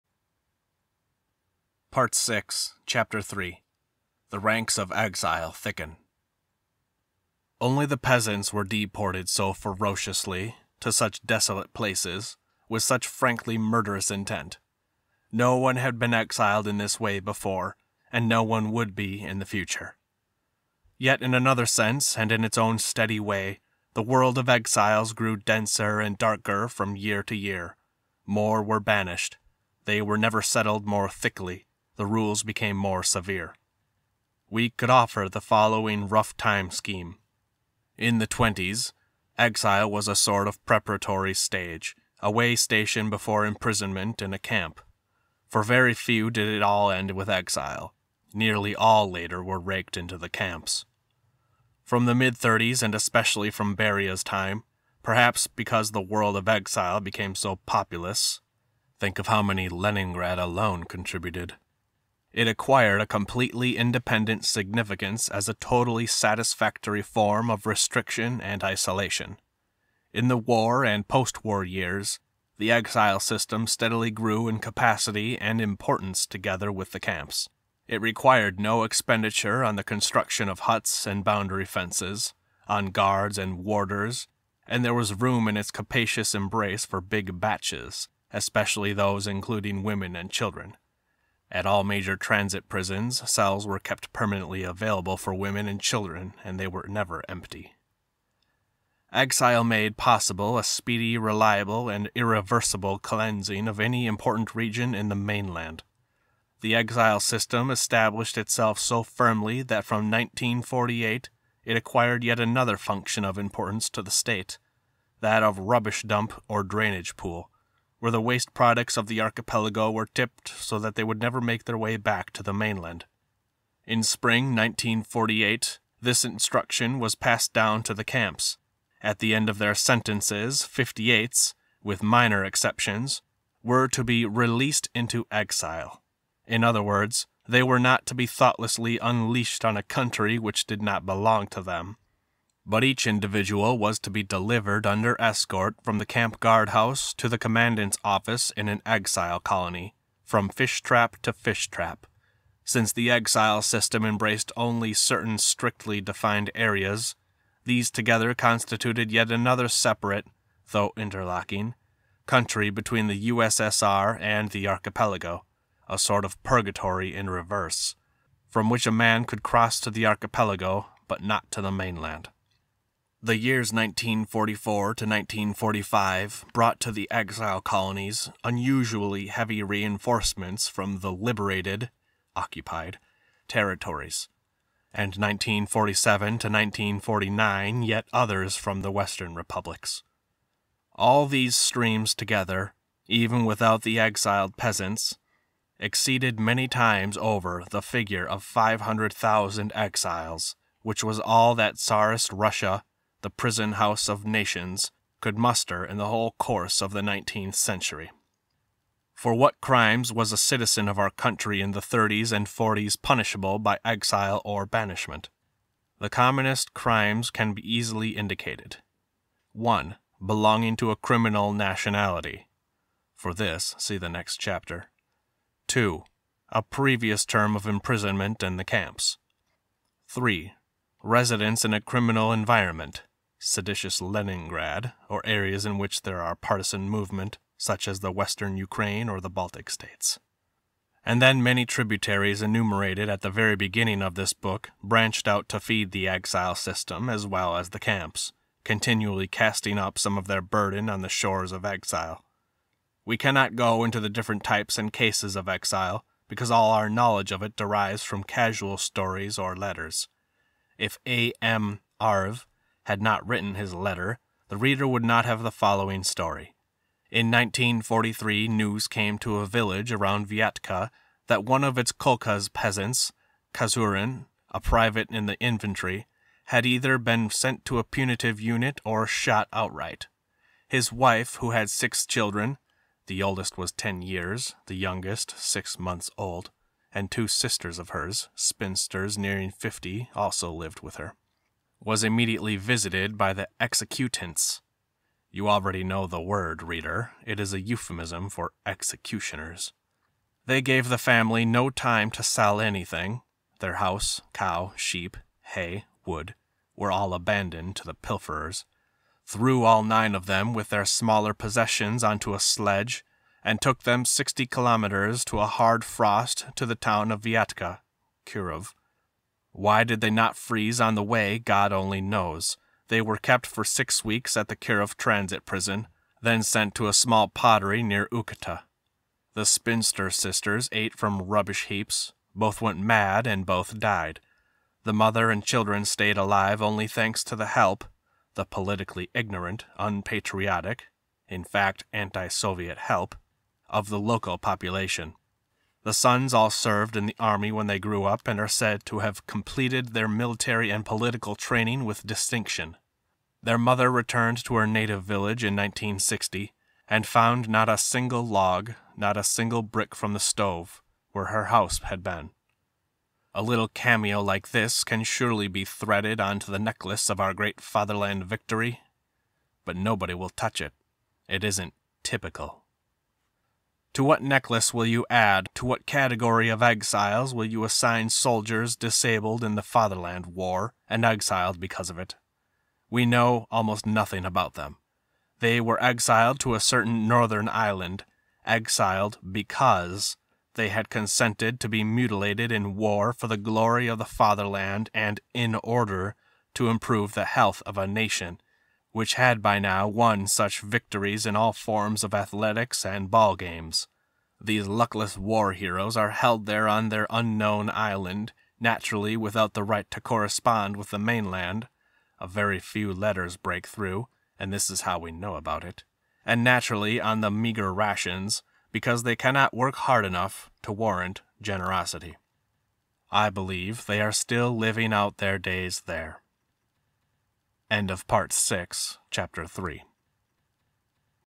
The Gulag Archipelago ABRIDGED - Part 6 Chapter 03 - The Ranks of Exile Thicken (fixed)
The Gulag Archipelago ABRIDGED
Part 6 Chapter 03 - The Ranks of Exile Thicken

I give my apologies to the no doubt often mispronounced Russian names and places. Additional apologies for sometimes mispronounced English words…

From this chapter:

"Only the peasants were deported so ferociously, to such desolate places, with such frankly murderous intent: no one had been exiled in this way before, and no one would be in the future. Yet in another sense and in its own steady way, the world of exiles grew denser and darker from year to year: more were banished, they were settled more thickly, the rules became more severe."

Aleksandr Solzhenitsyn
Translated by Thomas P. Whitney and Harry Willetts
Abridgement by Edward E. Ericson